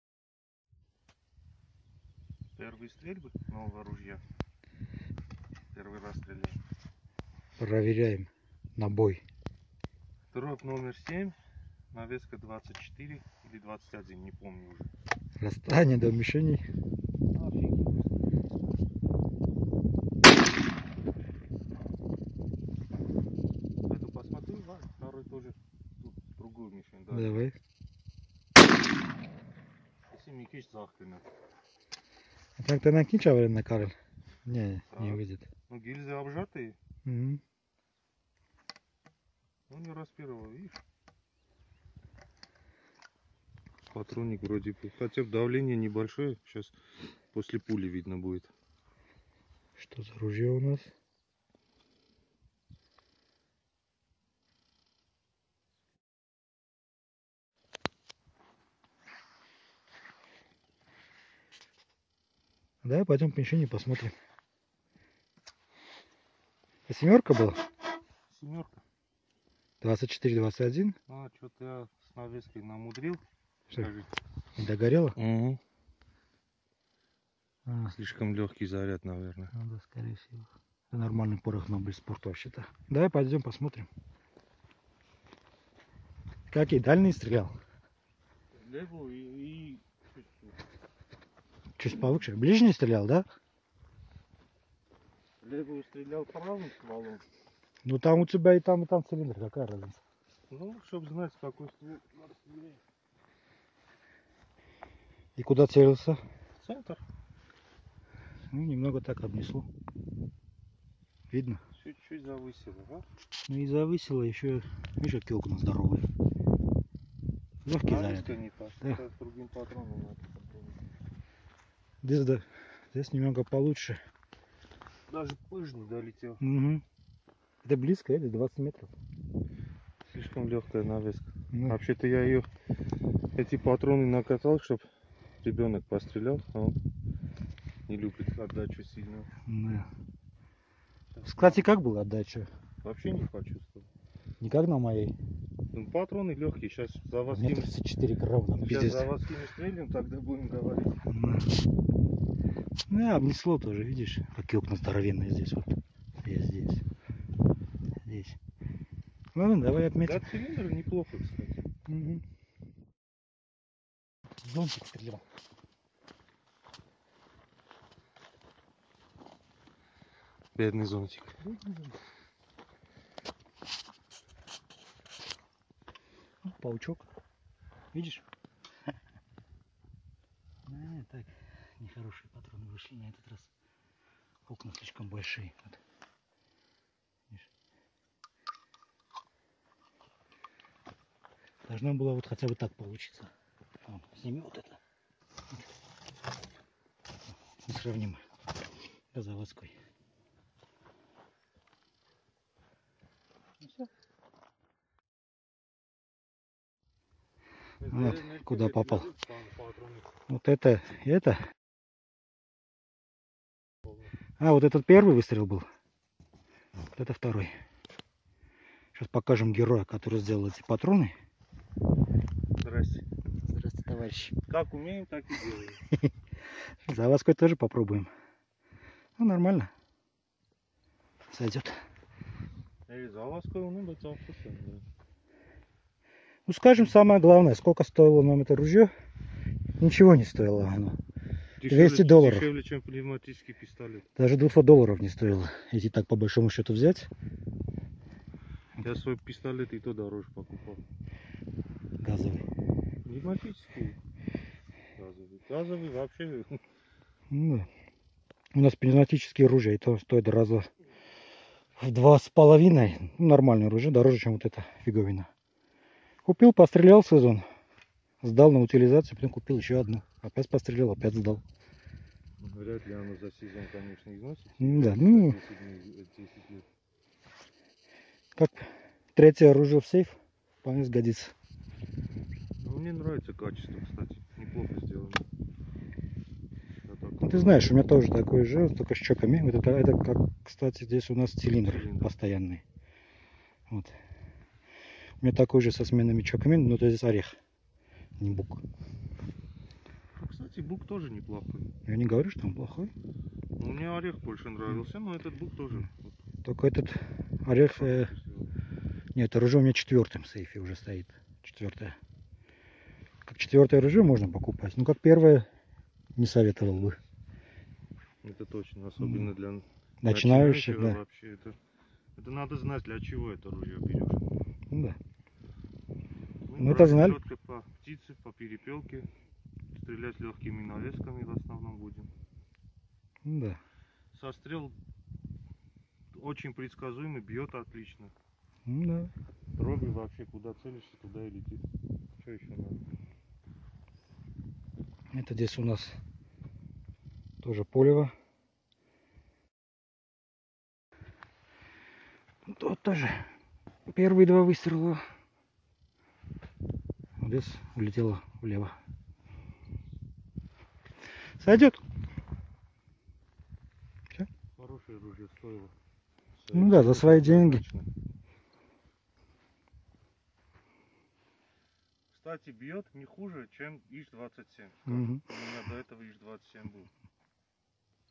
Ружье Мр-43, Remington 220
Первые стрельбы из нового ружья МР-43, экспортный вариант.